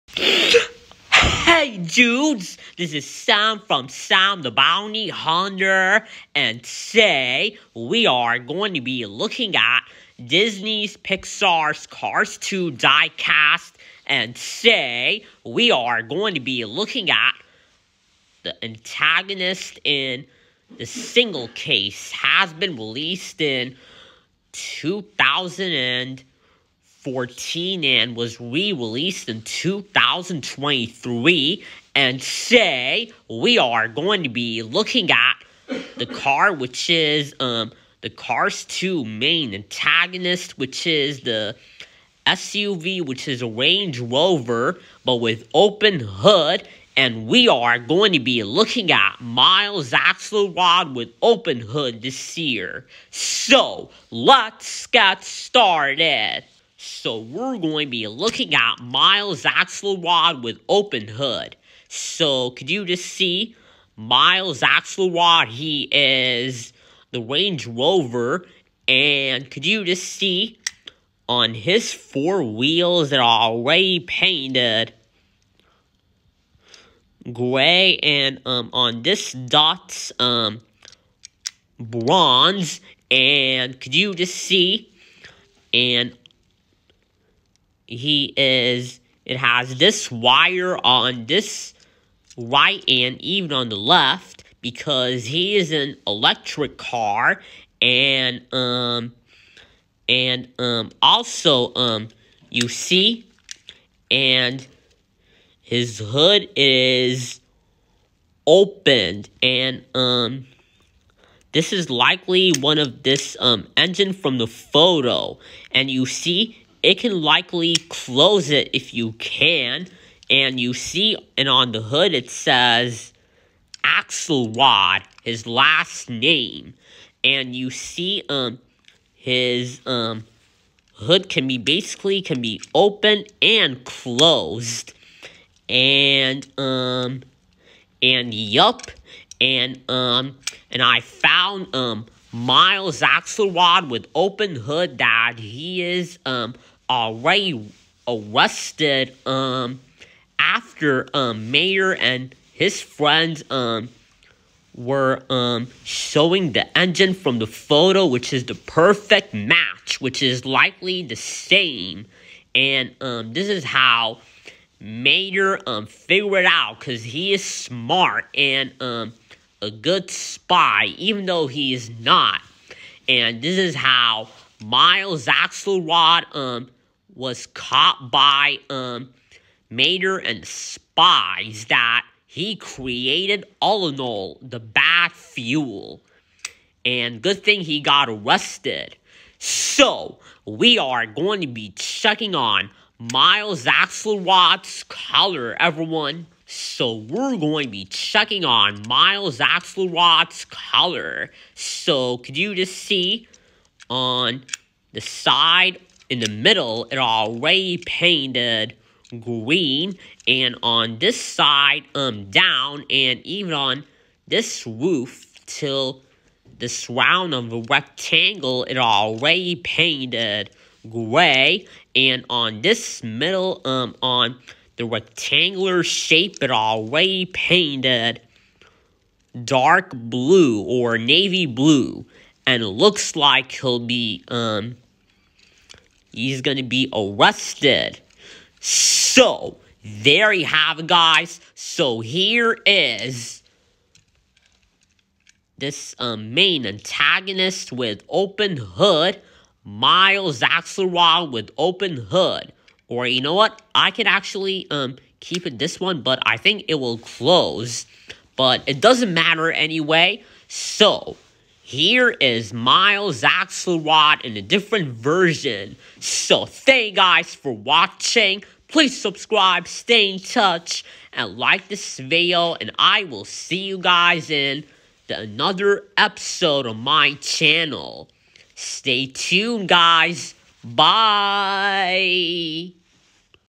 Disney Pixar Cars 2 Diecast Miles Axlerod With Open Hood
The villain with open hood in single pack has been released in 2014 and rereleased in 2023.